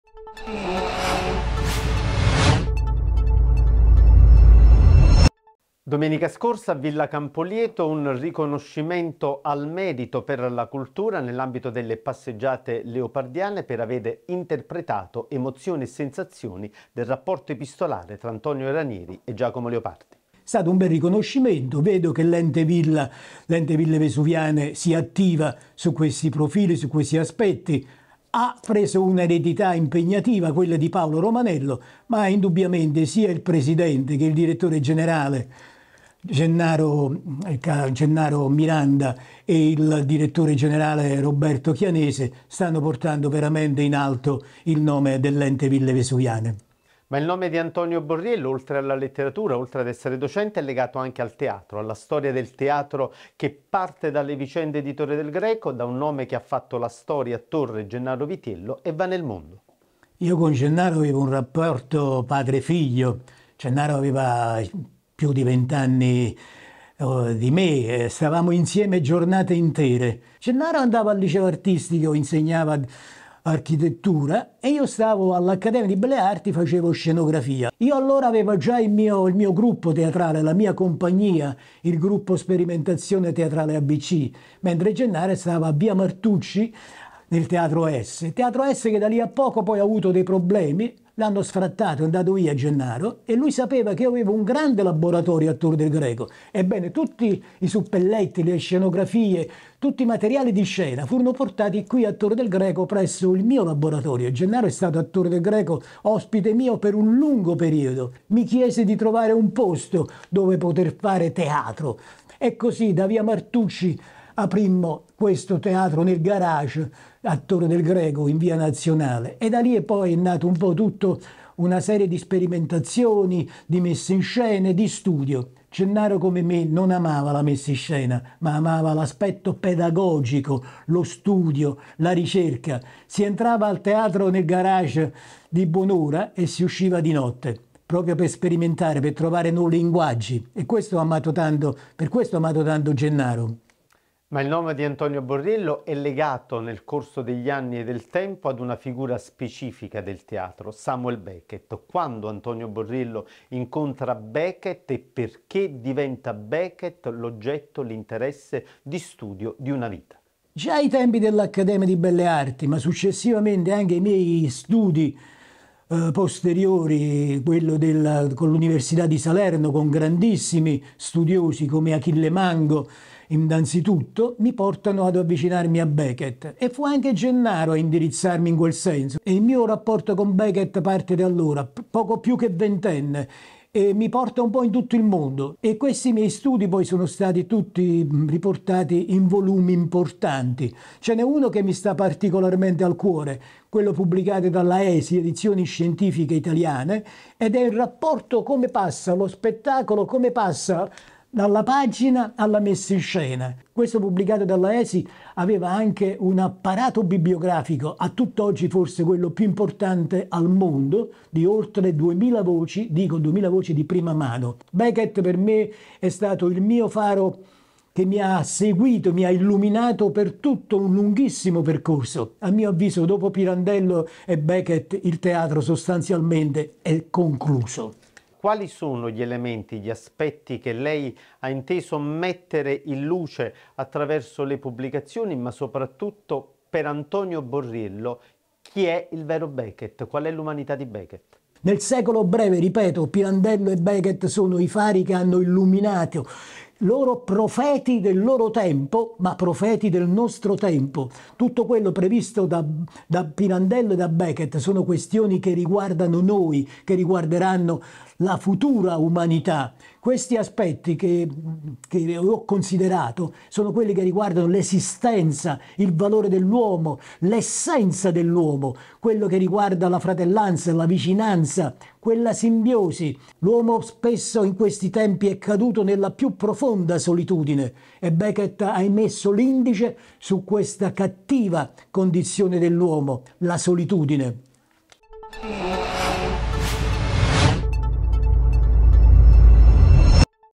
Passeggiate leopardiane, riconoscimento al professore Borriello
In occasione delle Passeggiate leopardiane 2024, conferito un attestato di ringraziamento al professore torrese Antonio Borriello per aver contribuito con impegno e passione alla realizzazione dell'evento.

"Sento di esprimere un doveroso ringraziamento alla Pro Loco, a Patrizia Porzio e a tutto il personale della Fondazione Ente Ville Vesuviane": sono le parole di Antonio Borriello.

La consegna si è tenuta nei giorni scorsi a Villa Campolieto.

Sono intervenuti il presidente Gennaro Miranda e il direttore generale Roberto Chianese.